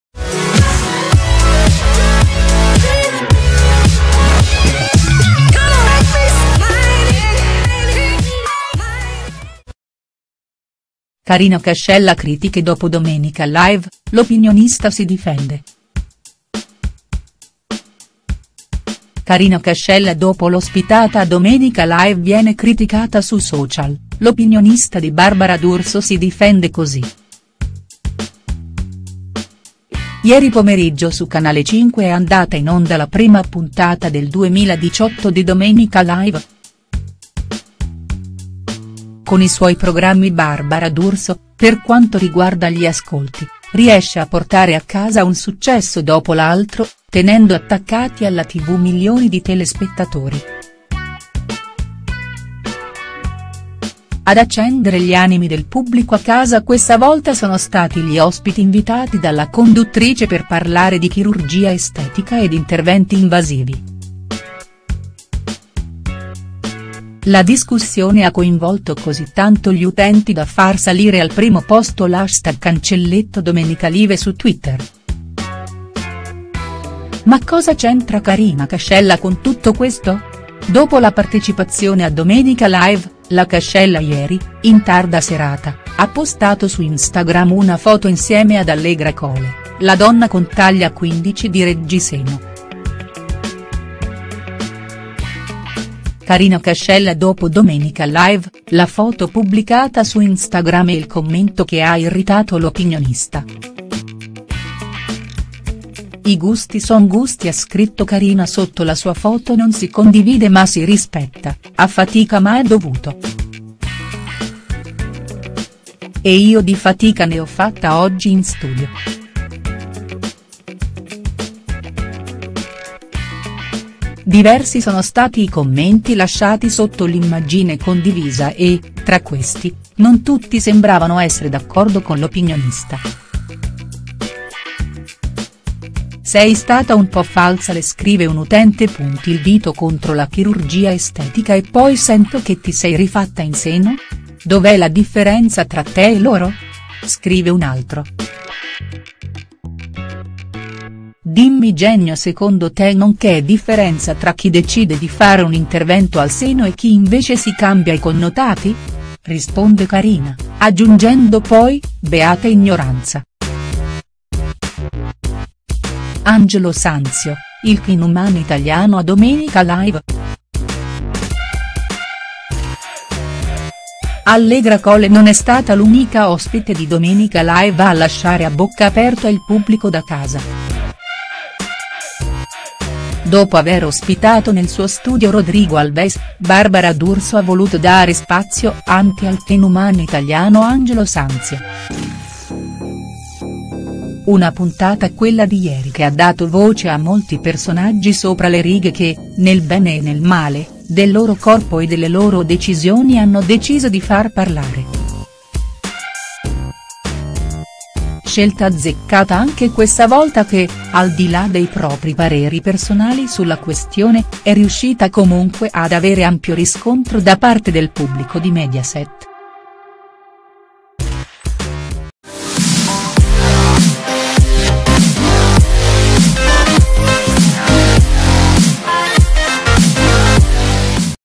Karina Cascella critiche dopo Domenica Live: l’opinionista si difende| STARS NEWS
Karina Cascella dopo l'ospitata a Domenica Live viene criticata sui social: l'opinionista di Barbara D'Urso si difende così.